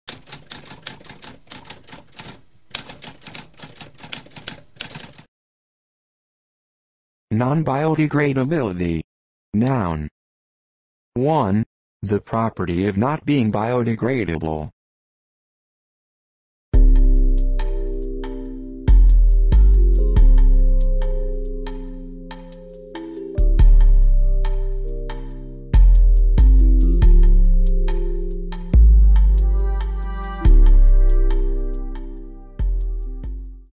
What does nonbiodegradability mean?
A spoken definition of nonbiodegradability.

Intro Sound:
Typewriter - Tamskp